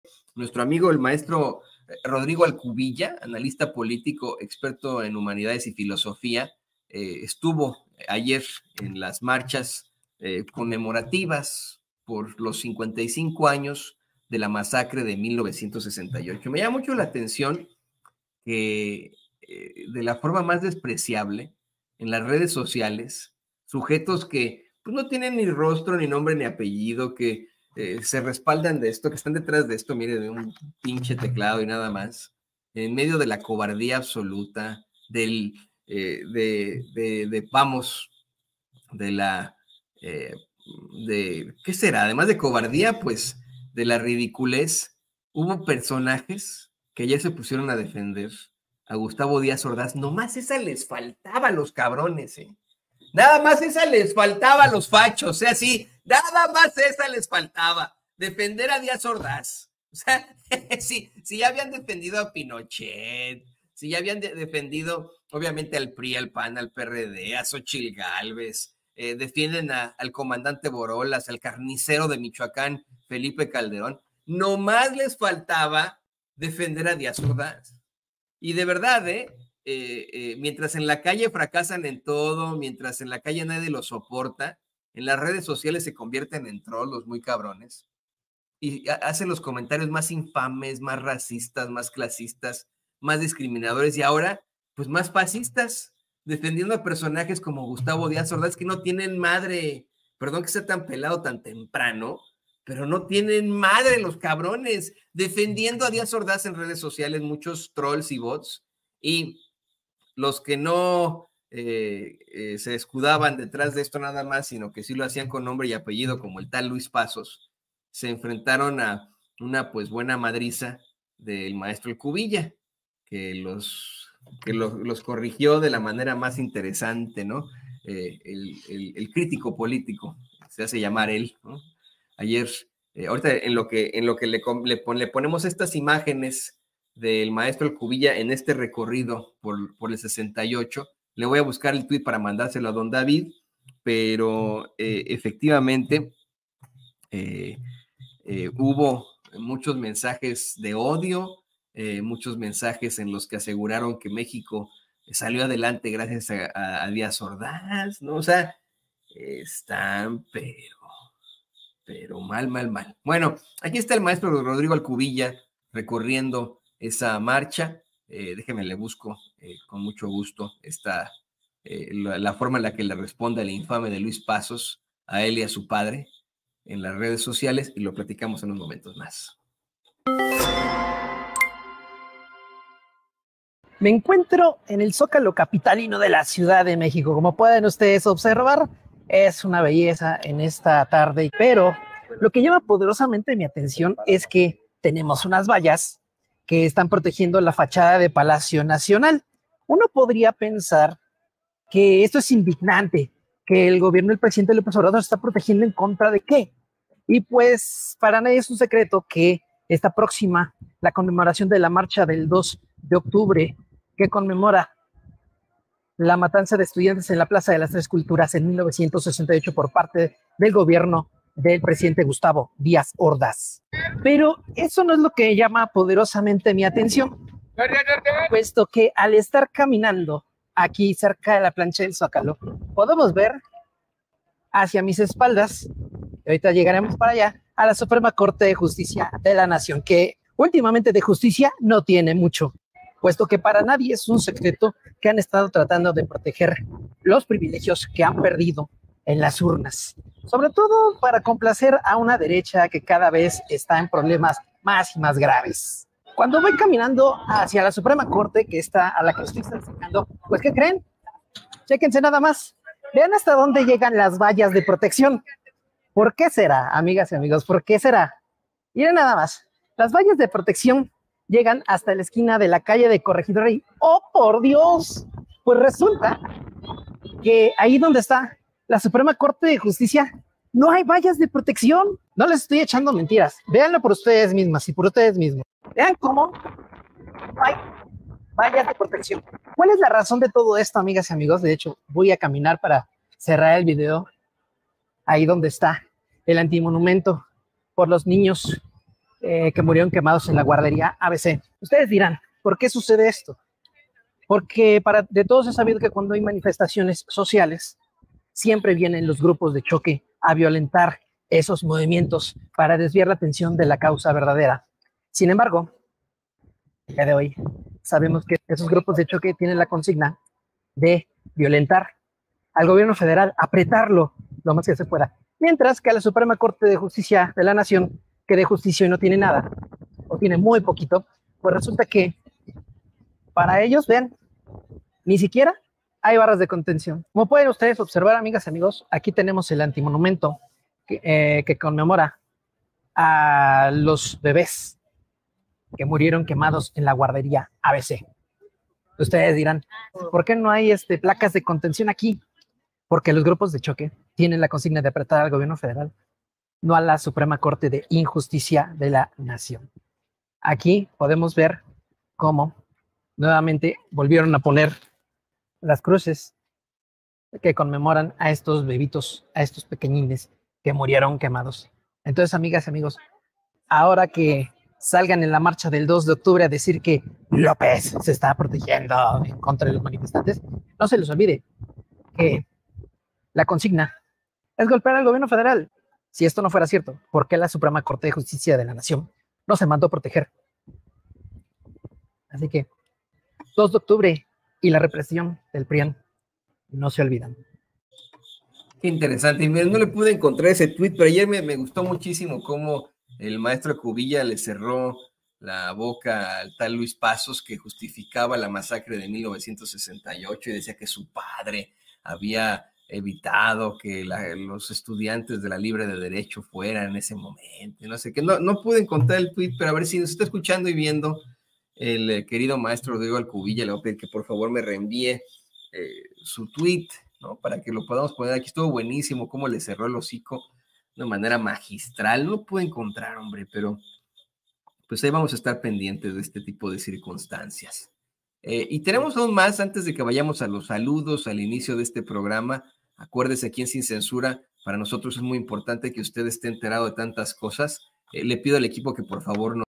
¿POR QUÉ LA SEDE DE LA CORTE NO FUE PROTEGIDA EN MARCHA DEL 2 DE OCTUBRE? MTRO. ALCUBILLA RESPONDE
El analista político, experto en humanidades y filosofía Rodrigo Alcubilla nos mostró cómo la sede de la Suprema Corte no fue protegida durante la marcha conmemorativa por el 55 aniversario de la m4s4cr2 de Tlatelolco ¿Por qué será? Pues porque la consigna era a manera de provocación a los grupos de choque y g0lp34r al gobierno federal.